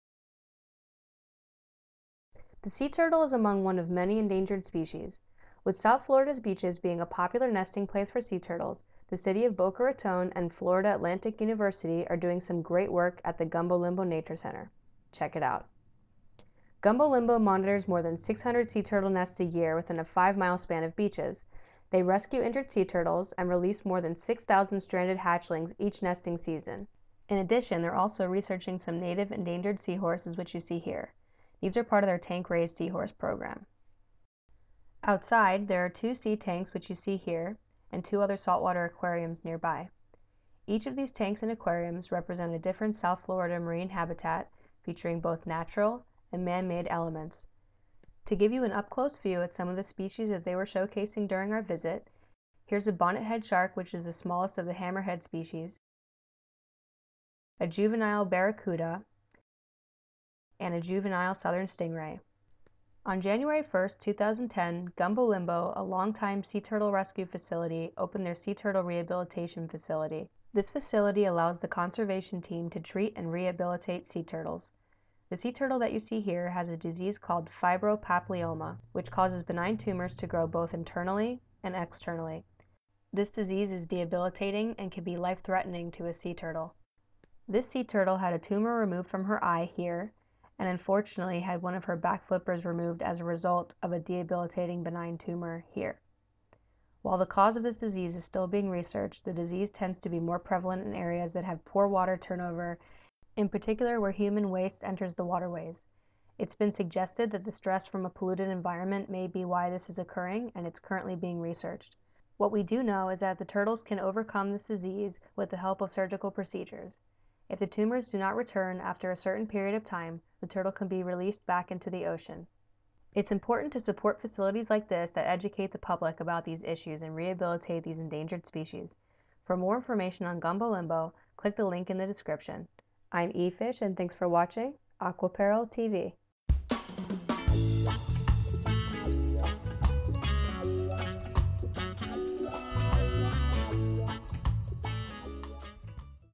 Gumbo Limbo Nature Center
On this episode EFish and Geoffrey go to the Gumbo Limbo Nature Center in Boca Raton, FL.